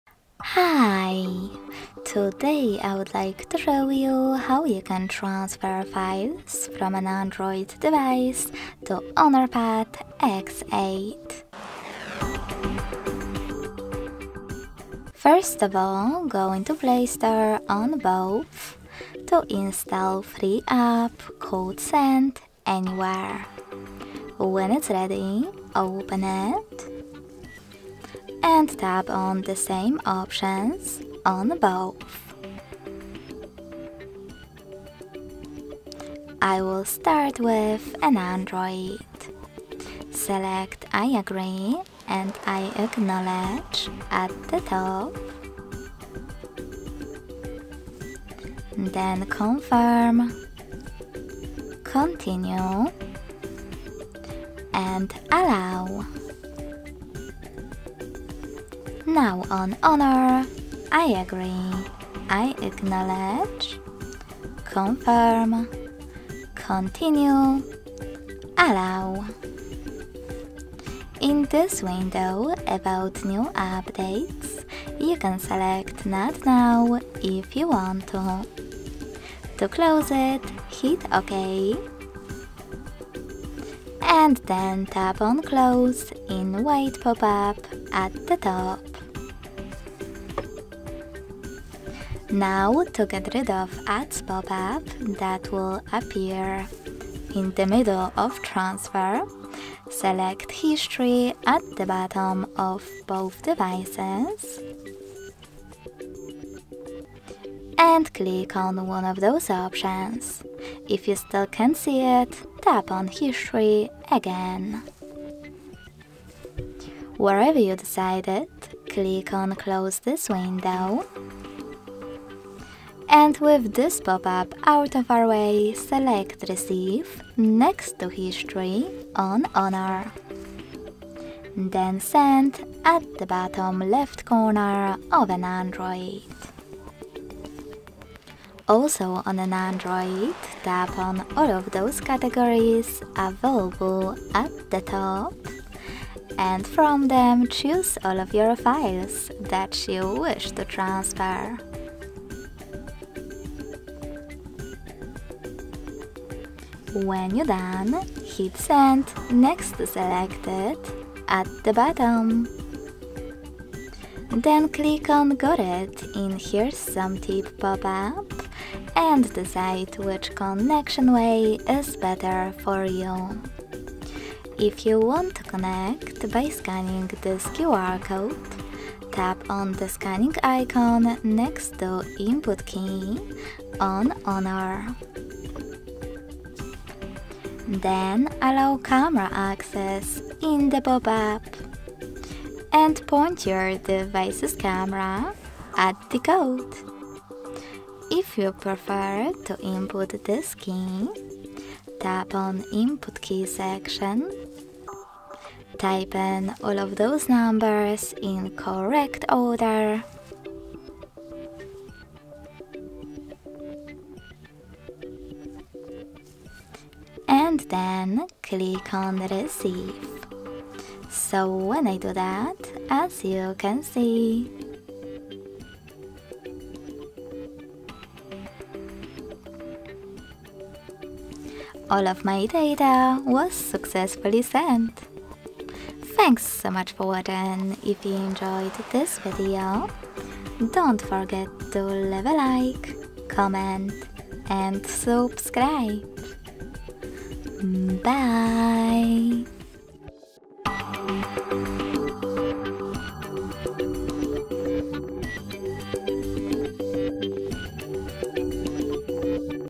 Transfer Files From Any Android Device to Honor Pad X8
Discover an easy and efficient method to transfer files from your Nokia device to the Honor Pad X8 in this step-by-step tutorial. Whether it's photos, videos, documents, or other data, you'll learn how to seamlessly migrate your content to your new device. Say goodbye to data loss and enjoy a smooth transition.

What are the common reasons for users wanting to transfer files from a Nokia device to an Honor Pad X8?
Are there any special tools or apps required for this file transfer process, or can it be done natively?
Can you provide a detailed guide on how to initiate and complete the file transfer step by step?
Are there any precautions users should take to ensure their data is safely transferred without any loss or corruption?